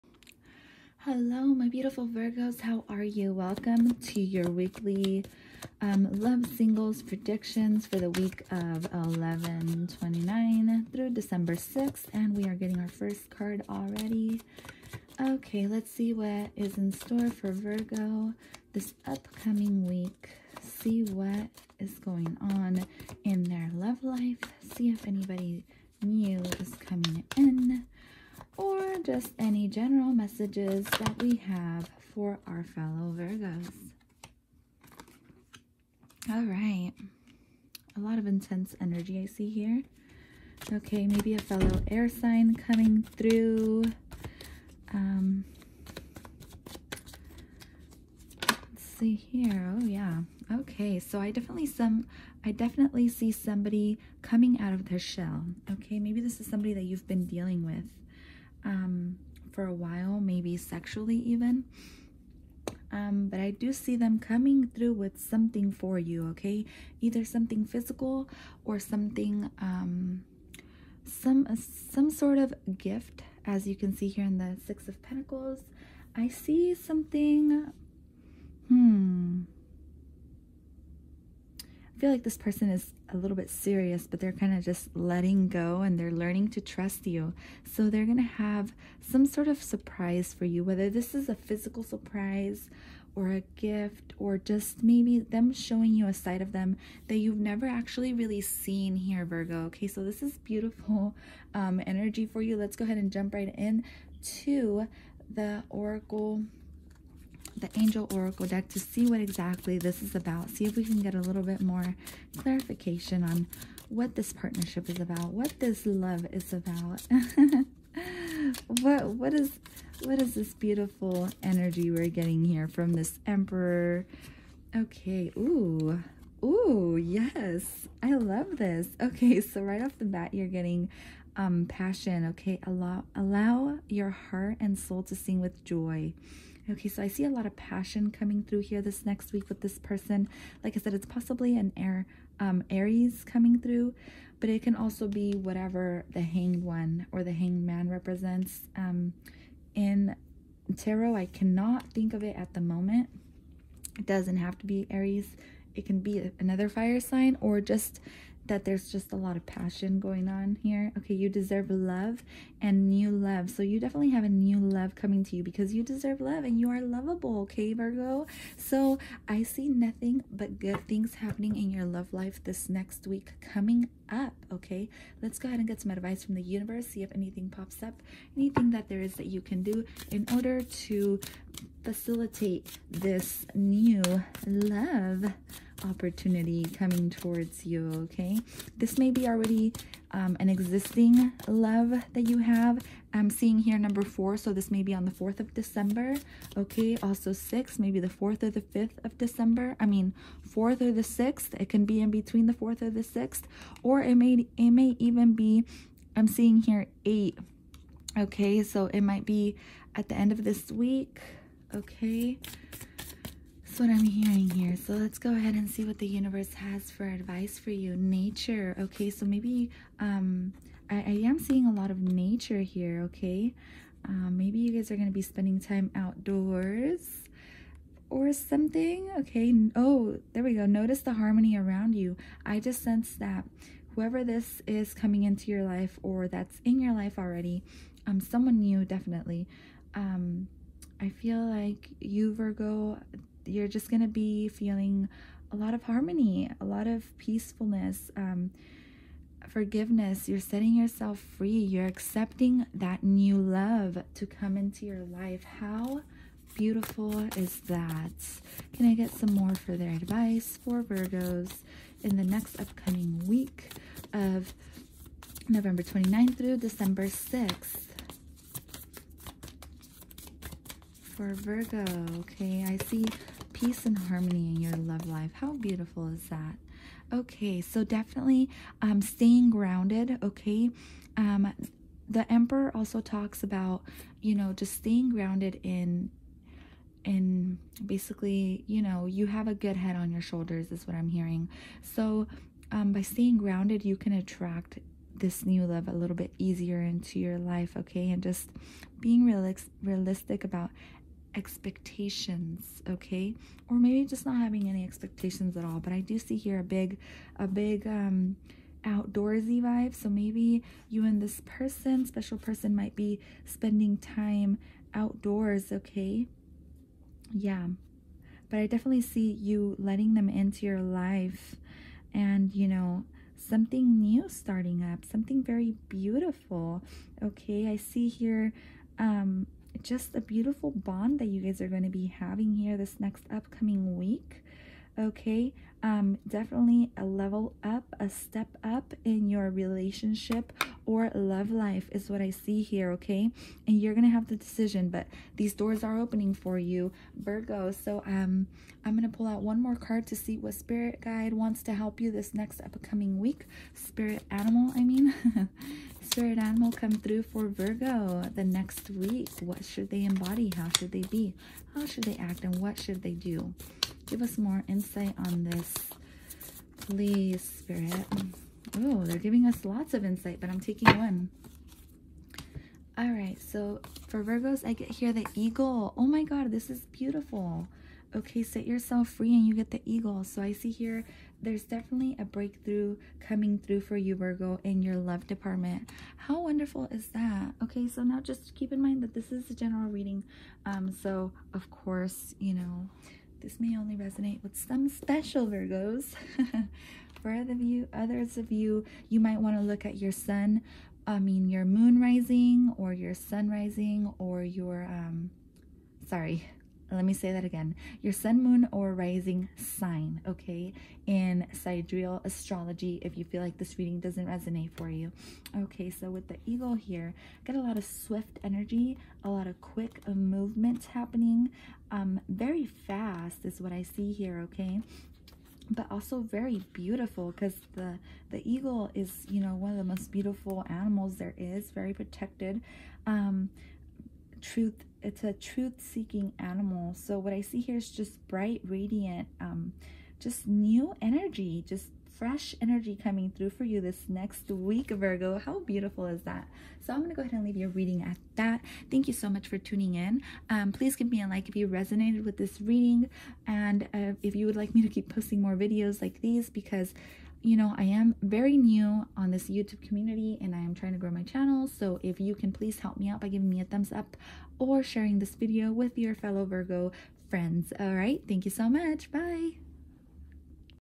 ♍️ Look who’s coming in 👀 VIRGO LOVE SINGLES TAROT READING 11/29-12/6
IS THIS the one VIRGO???☎️❤️⏳

**GENERAL READING** may not resonate with everyone

TAROT READER TIP: I suggest watching sun, moon, and rising sign. To figure out ur sun moon and rising sign, go check out ur birth chart on a birth chart calculator using SIDEREAL ASTROLOGY for more accurate readings/results.